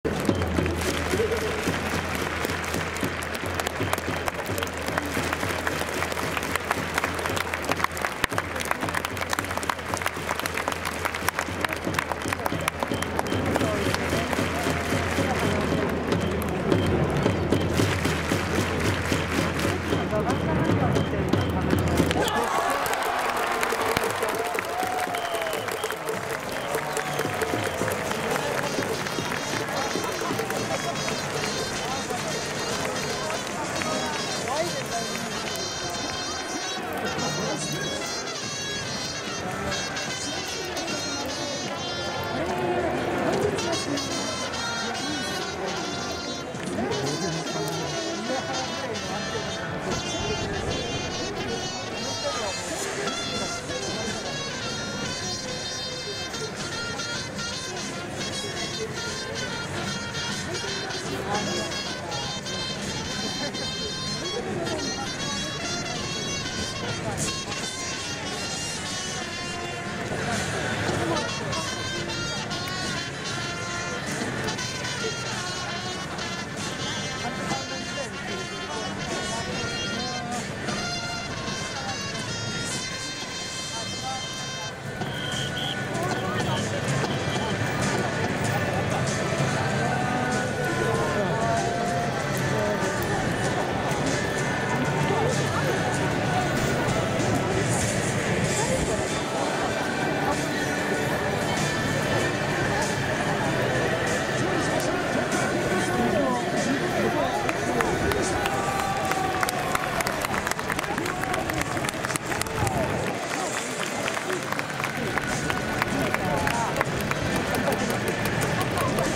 つば九郎、勝利した選手たちを出迎える 2022/6/4
2022/6/4 神宮球場
東京ヤクルトスワローズ VS 埼玉西武ライオンズ

見事な勝利でした